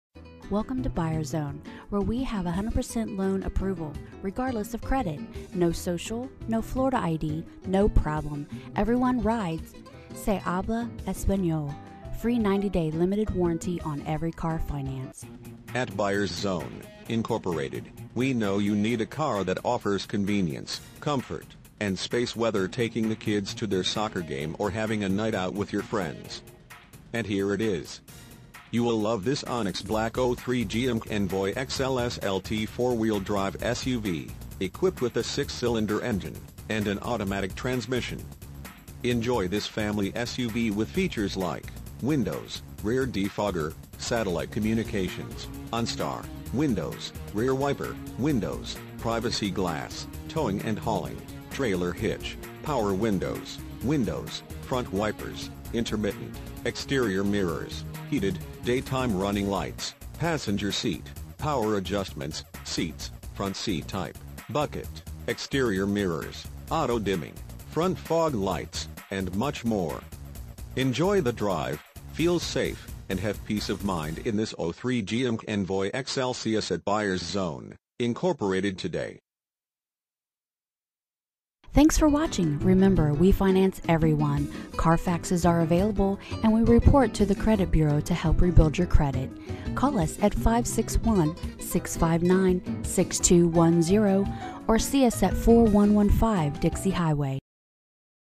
2003 GMC Envoy XL Buyers Zone, Inc. West Palm Beach, FL 33405
GMC Envoy XL At Buyers Zone, Inc., we know you are looking for a vehicle to solve everyday task. Whether loading the kids soccer equipment or having a night out with your friends. You will have the room you need making those everyday task an ease to accomplish. Imagine driving this Onyx Black Gmc Envoy Xl SLT 4WD 4dr SUV, equipped with a 6 Cylinder engine and an automatic transmission.

Buyers Zone, Inc.
4115 South Dixie Highway
West Palm Beach, Florida 33405